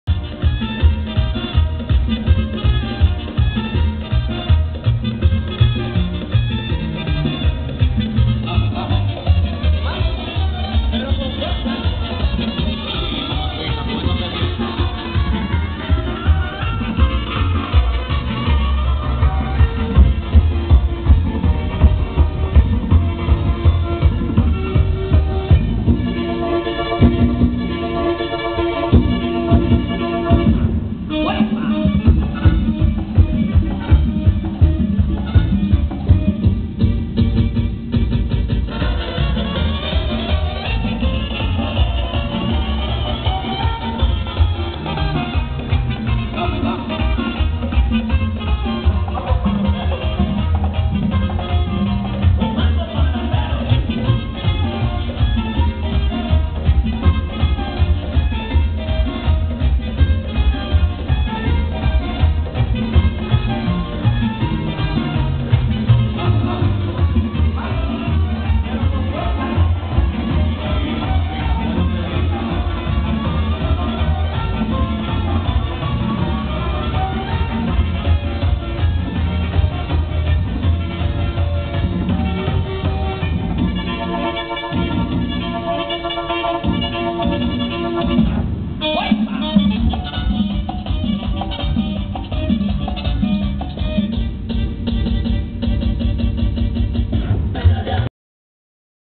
More of My Decorations
More of my decorations. "Noches Tropicales 10/28/2010 in Asociacion Borinquena, Orlando FL.